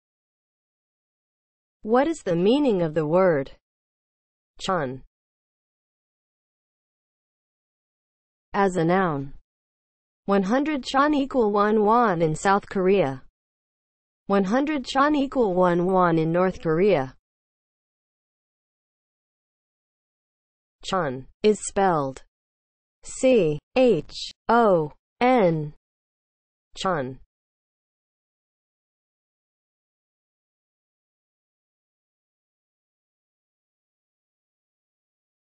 What is the meaning of the word CHON?
Definition and spelling of the word CHON.

(00:00) Intro
(00:07) As a noun
(00:19) Spelling

As a noun (00:07):
• 100 chon equal 1 won in South Korea
• 100 chon equal 1 won in North Korea